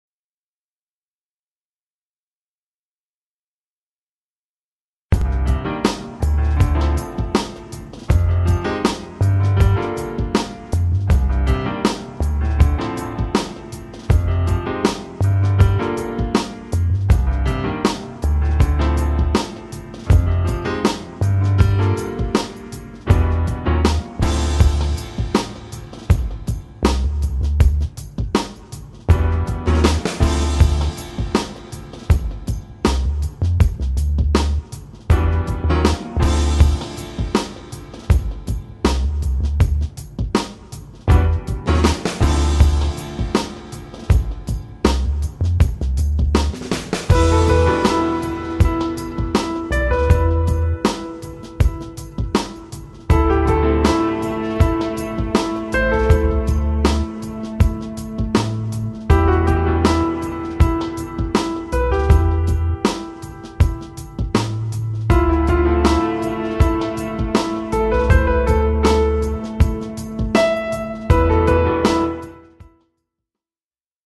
Renault kadjar occasion visible à Bayeux présentée par Renault bayeux
Cette Renault kadjar occasion disponible à Bayeux dans le 14 est mise en vente par Renault bayeux - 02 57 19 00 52 - tous les détails sont